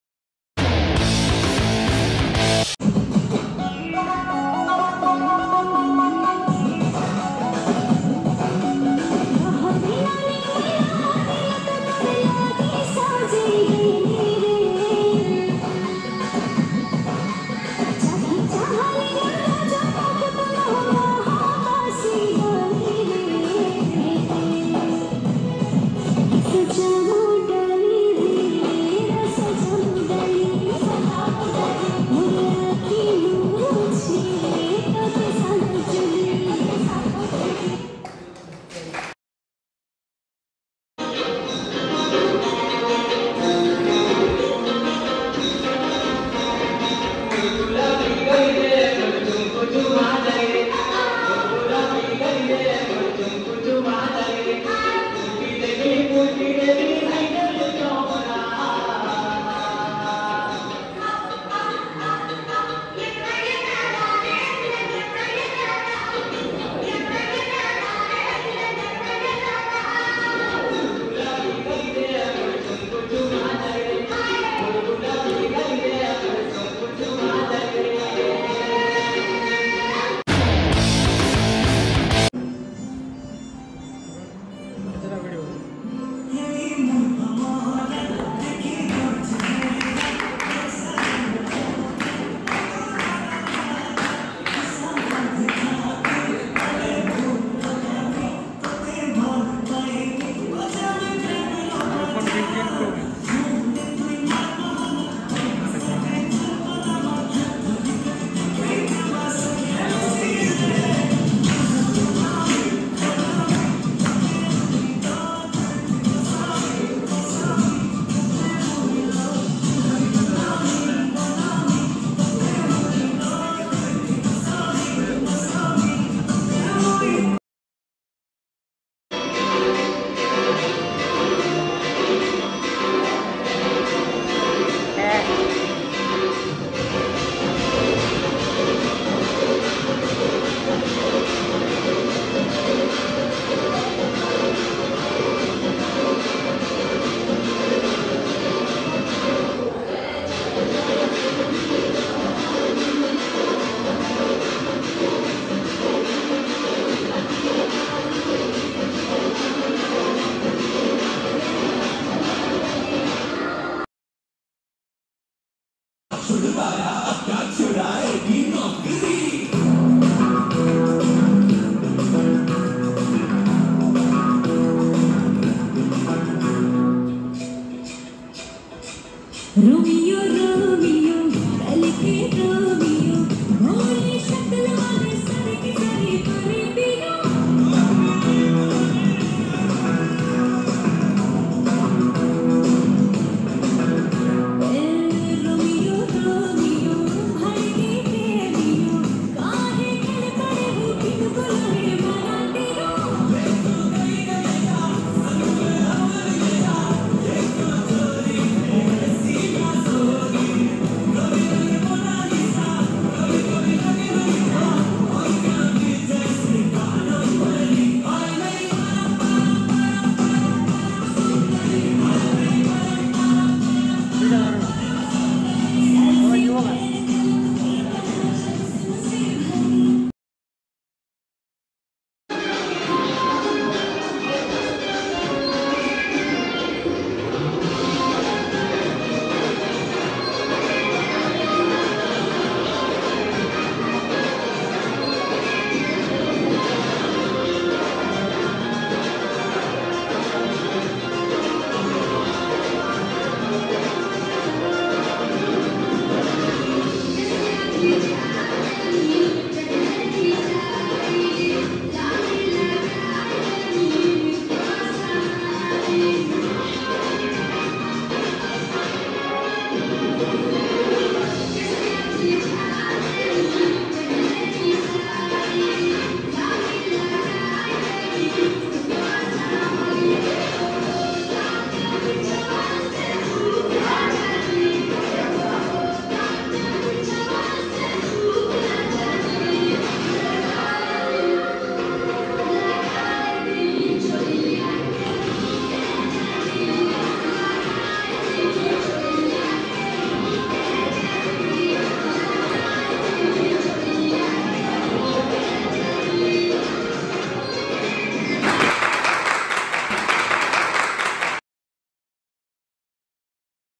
Dance Challenge girl vs boy||Sonepur Raj College ||Commerce day function
Dancer -Srusti & Sumanta

sambalpuri dance
hindi dance